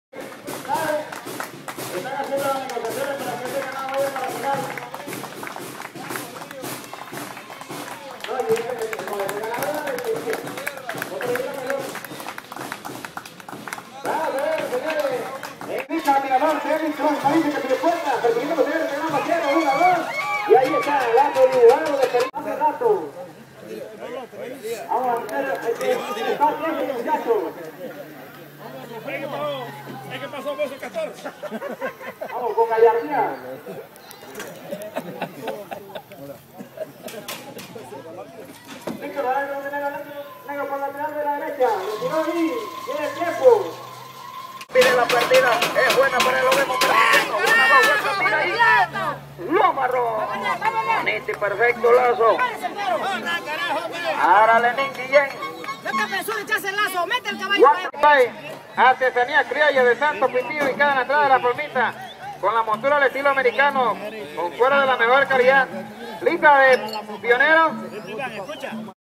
A roping in Panama
This is a roping held in Panama.  The ropers compete in teams, no money